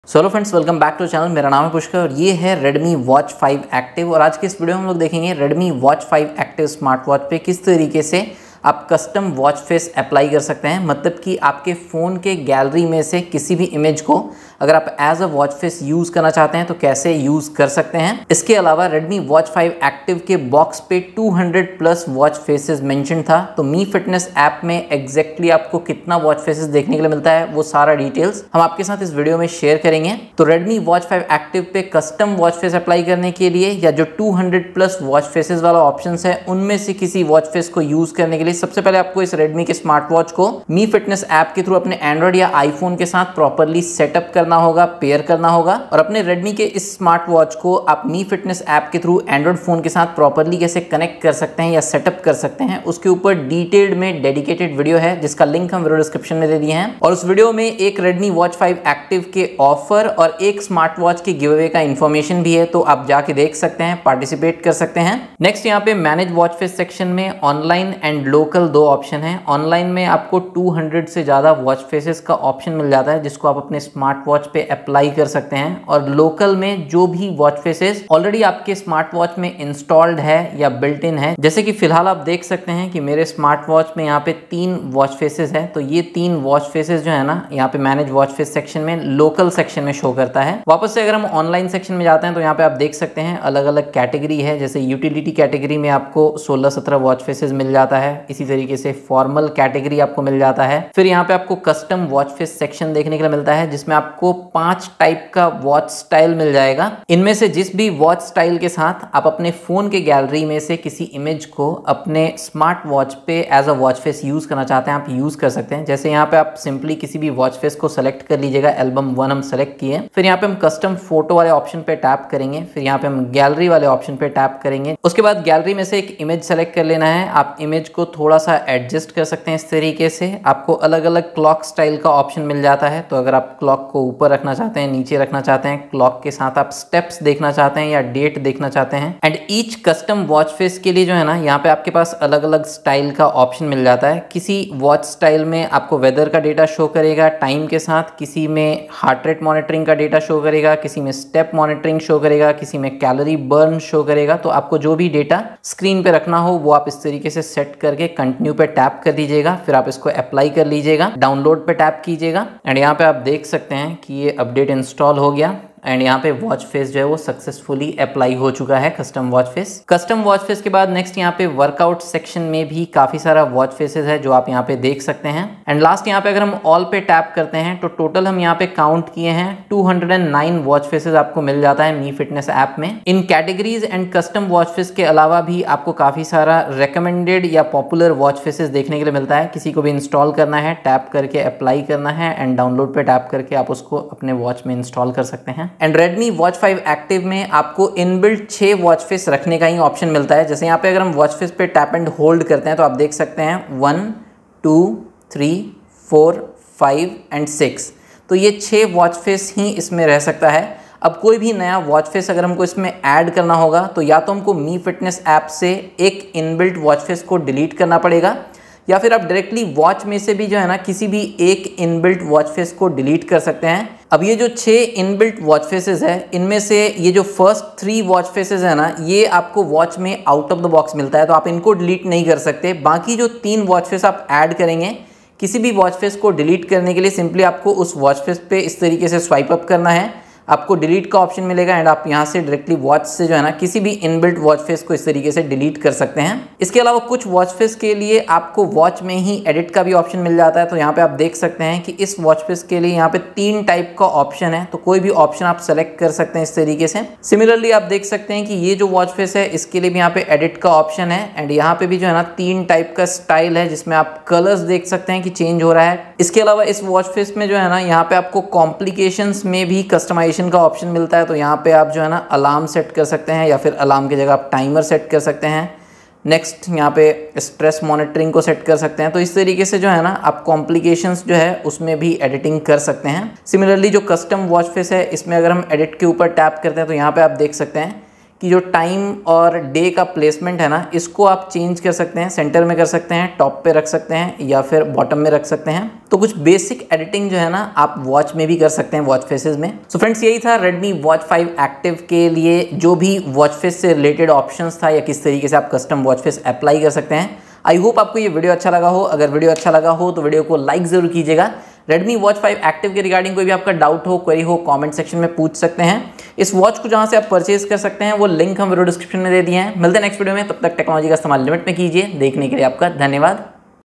Redmi Watch 5 Active - Custom Watch Face & Settings | Mi Fitness App
Redmi Watch 5 Active

This video is about the Redmi Watch 5 Active Custom Watch Face - How to apply custom watch face, and watch face settings in Mi Fitness App.

Redmi Watch 5 Active key Specification -
2-inch AMOLED display,
140 Sports mode,
bluetooth V5.3,
SPO2 Monitor, Heart Rate Monitor, sleep monitoring, Stress Monitoring e.t.c
Compatible app - MI fitness

Gadgets I Use to Record & Edit Video -